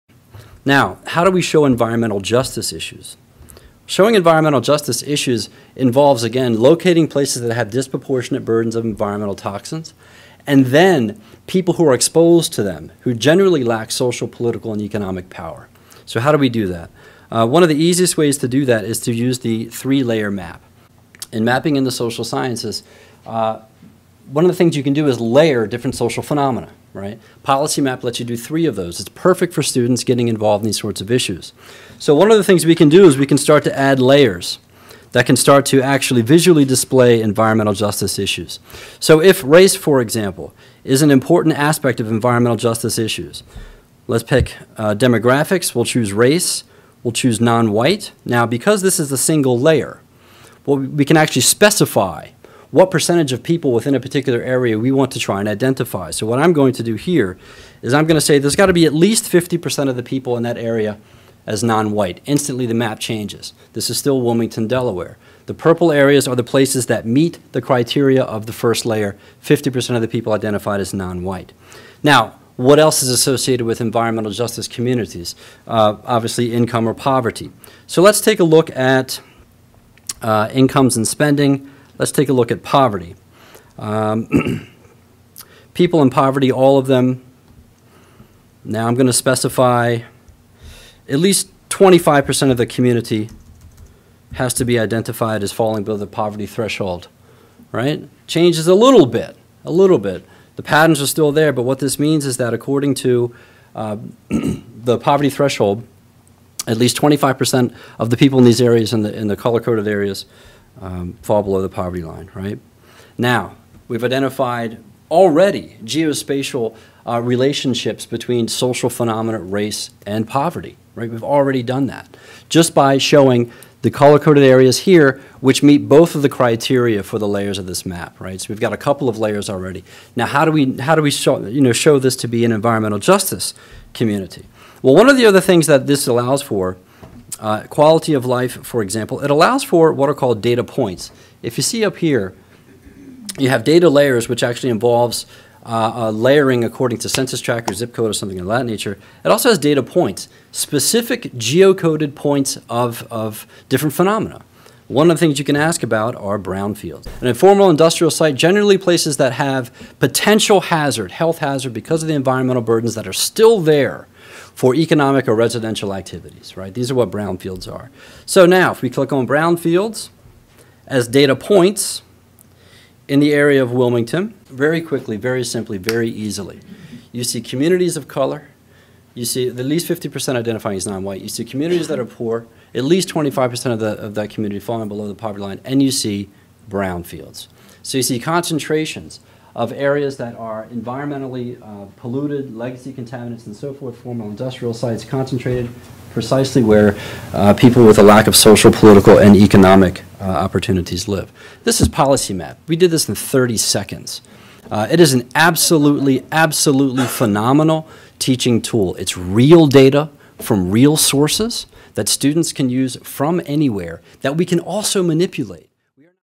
Using PolicyMap to Teach Environmental (In)Justice
From University of Delaware Professor Victor Perez:

"I presented on using PolicyMap to examine environmental justice issues in local Delaware.  Studying environmental justice communities with maps allows students to see several related social phenomena, including race and socio-economic status, layered with specific geographic areas that have disproportionate levels of environmental burdens.  The spatial patterns of race, socio-economic status, and pollution are not a coincidence, and have deep roots in industry, poverty, housing, race, and discrimination.  This presentation highlights one example drawn from the planned curriculum for a general education course on integrating spatial visualizations into social science analyses."